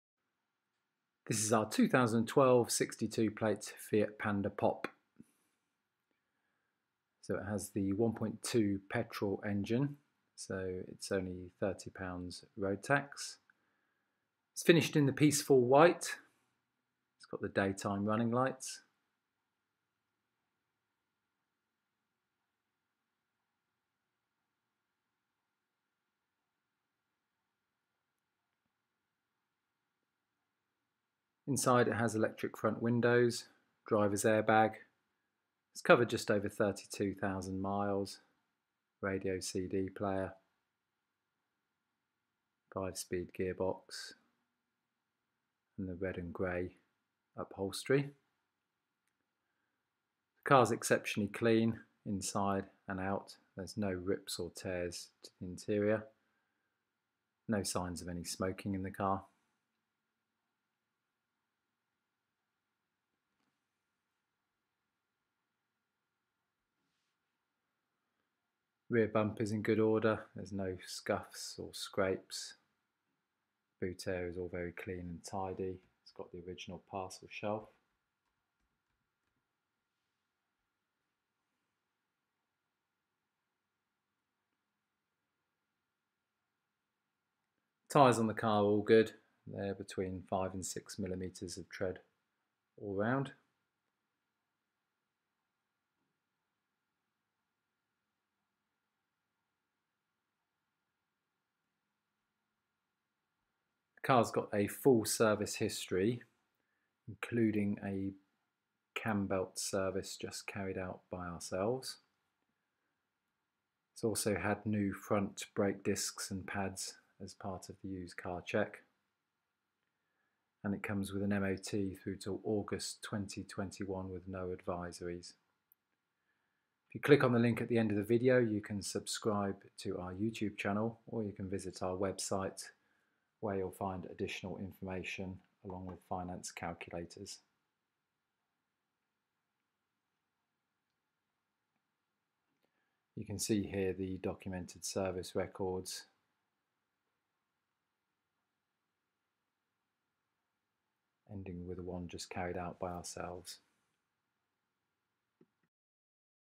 2012 Fiat Panda Pop White
2012 (62) Fiat Panda Pop Peaceful White 1.2 for sale at Norfolk Cars